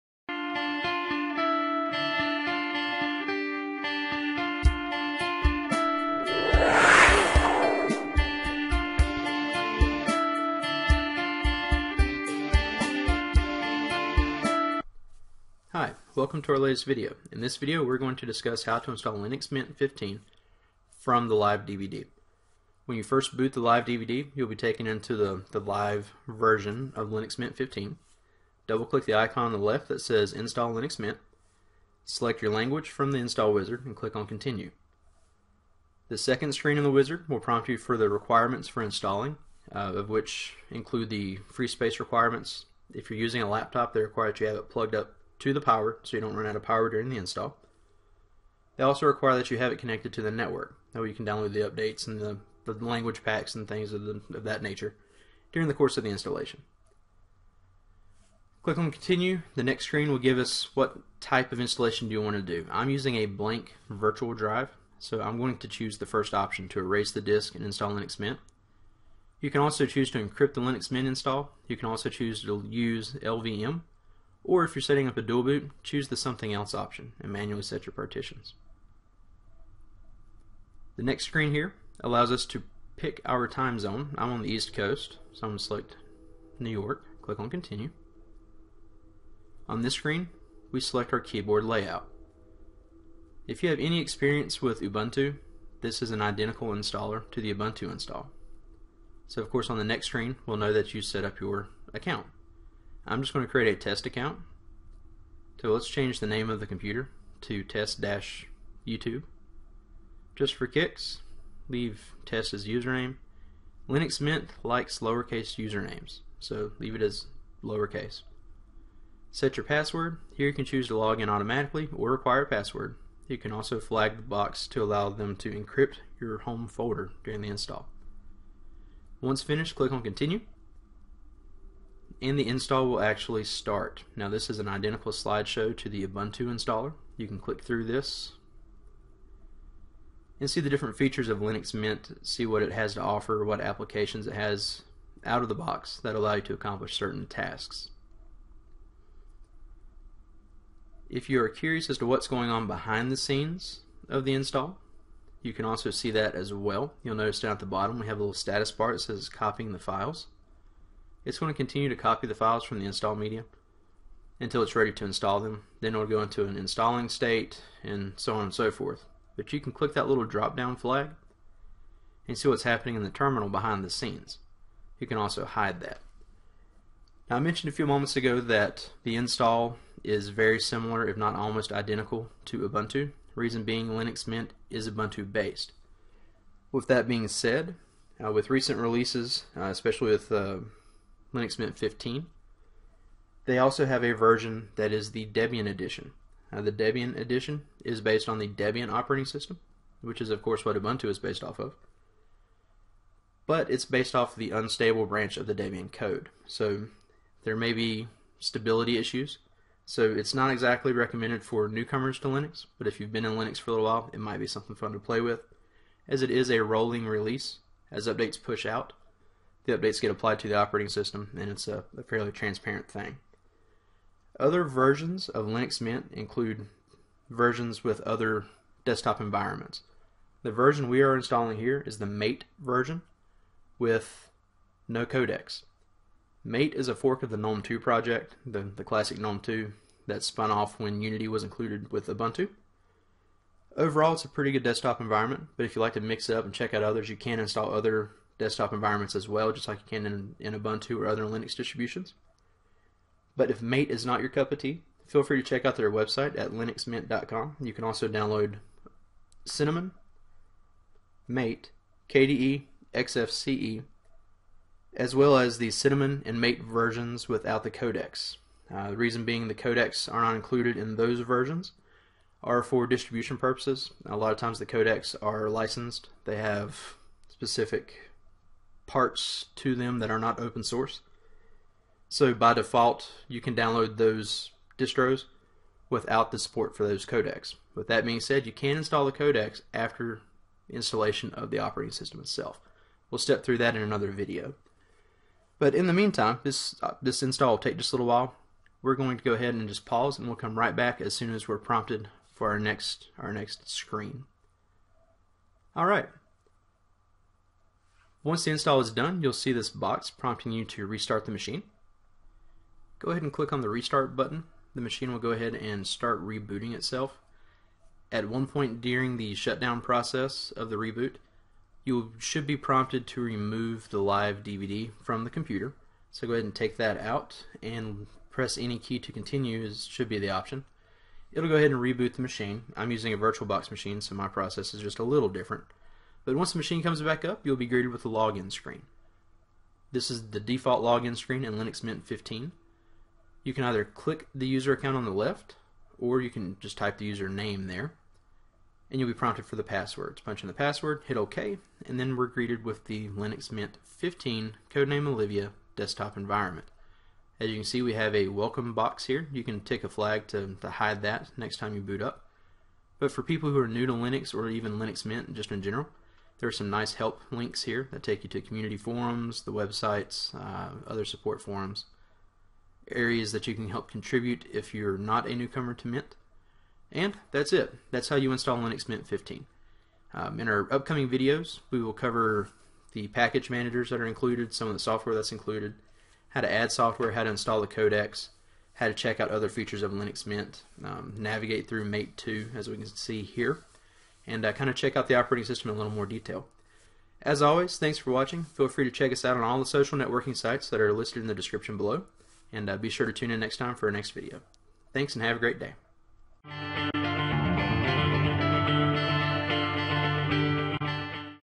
Install Linux Mint 15 Olivia
In this video we are going to discuss how to install Linux Mint 15 'Olivia'.  In this video I am using the 'No Codecs' version with the Mate Desktop Environment and am installing Mint on a VirtualBox machine.  The steps are the same for the physical computer as well.

Things you will need to follow along:
- Linux Mint 15 Live DVD
- Empty Hard Drive/Enough Free Space for the Install.

To get a copy of the Linux Mint 15 Live DVD please
Once there, pick the version of Linux Mint that looks interesting to you, the install steps should be pretty much the same between them all.

Also, be sure to find us on all of the major social networking sites (the links are listed below):